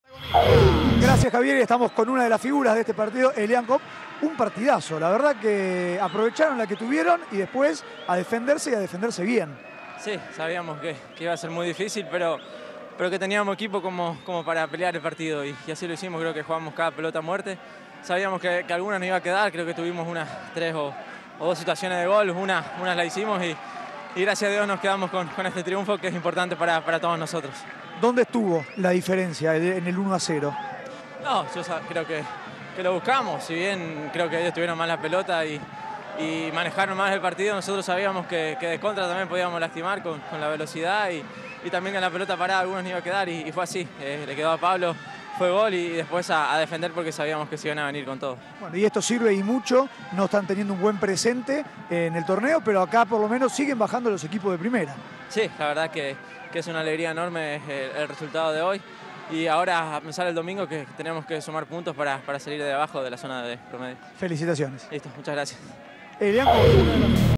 "Una nos iba a quedar". Argentinos 0 - Guaraní 1. 16avos de final. Copa Argentina 2015. FPT
Luego del triunfo ante Argentinos, Elian Kopp admitió que "sabíamos que iba a ser un partido difícil pero también que teníamos equipo para pelearlo". Además, consideró que "lo buscamos a pesar de que ellos hayan dominado los 90 minutos".